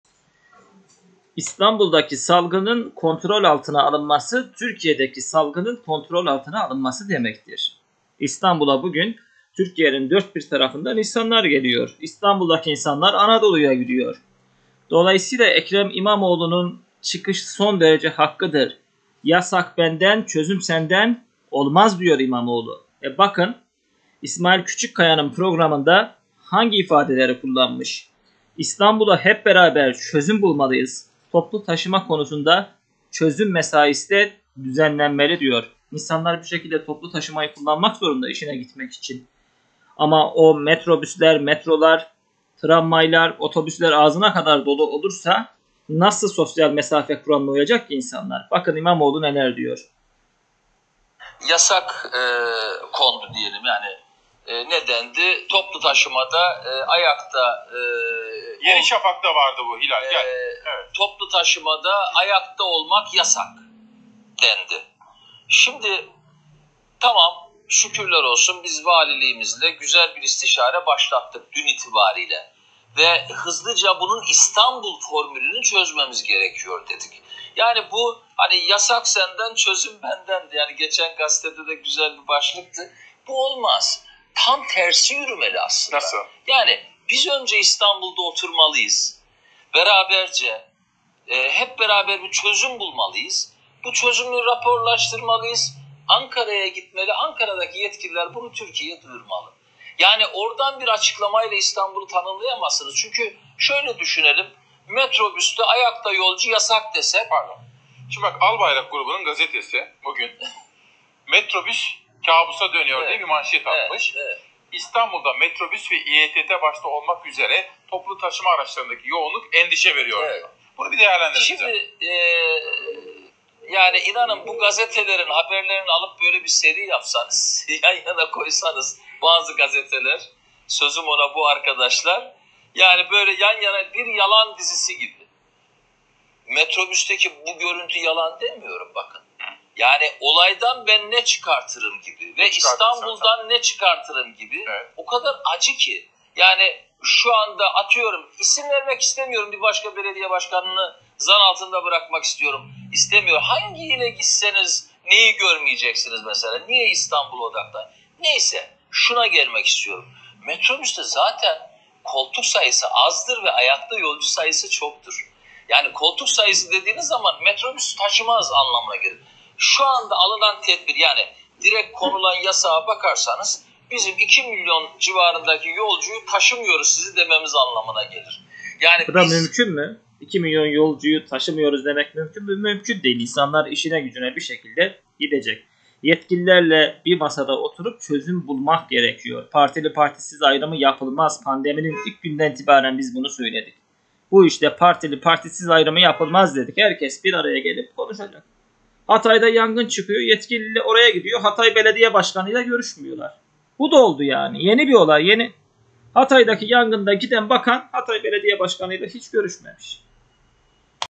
FOX TV'de Ekrem İmamoğlu çileden Çıktı Yasaklarla İlgili Konuştu..
İstanbul Büyükşehir  Belediye Başkanı Ekrem İmamoğlu FOX TV'de İsmail Küçükkaya'nın konuğu oldu. Yasaklarla ilgili önemli bir çağrıda bulundu..